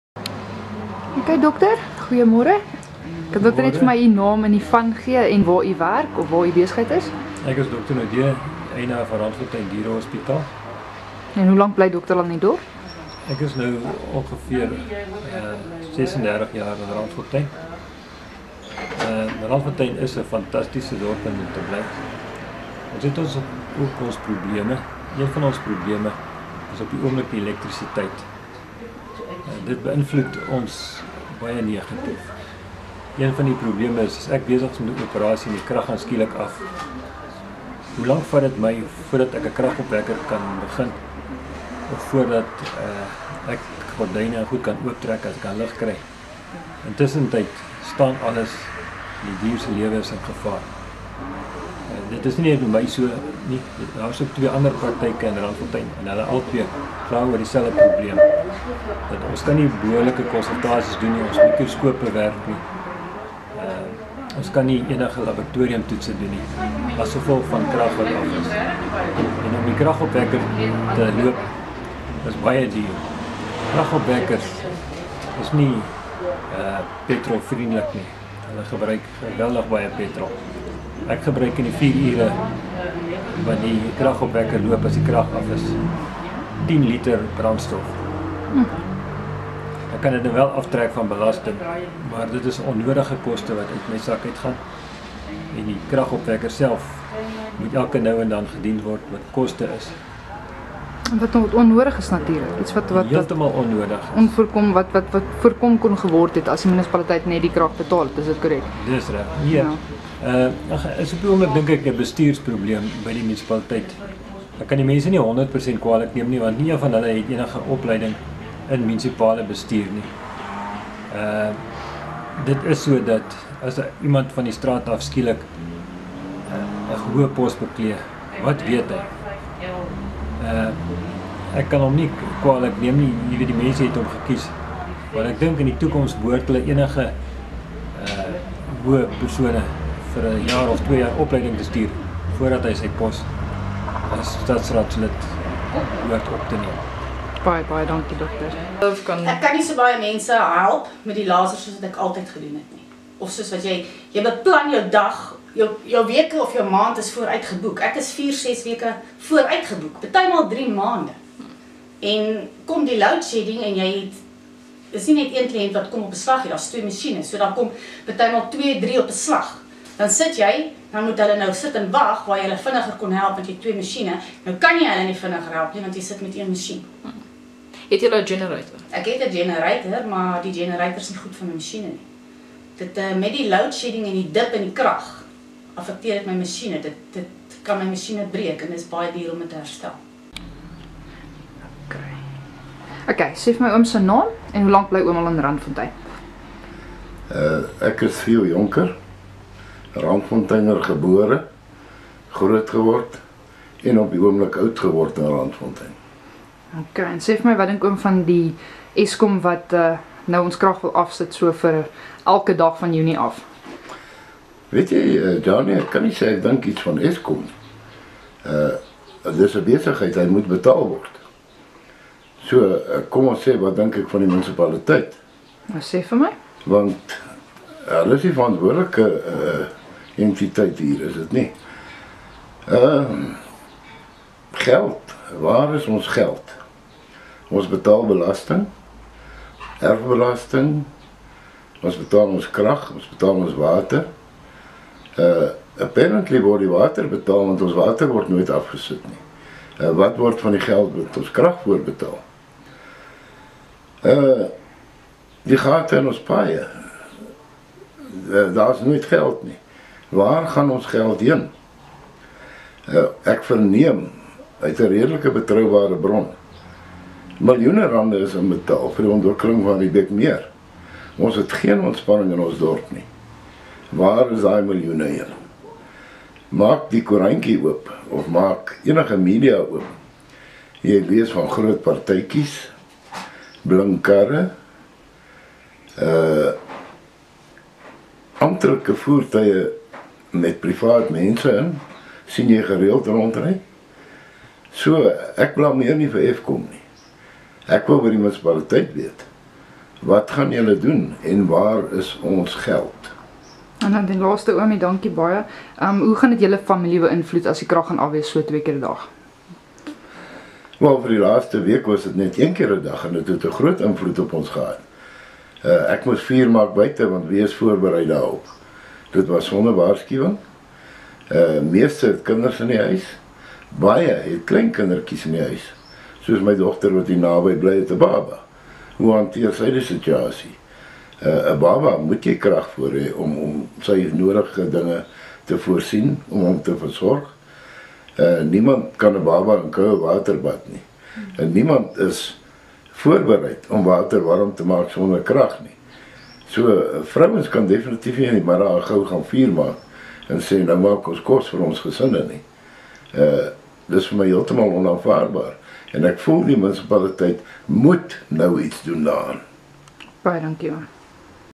Randfonteiners oor Eskom wat die krag in Junie gaan afsny
Eskom dreig om die krag in Randfontein komende Junie 2015 af te sny oor die munisipaliteit nie die kragvoorsiener betaal nie. Inwoners gee hul mening.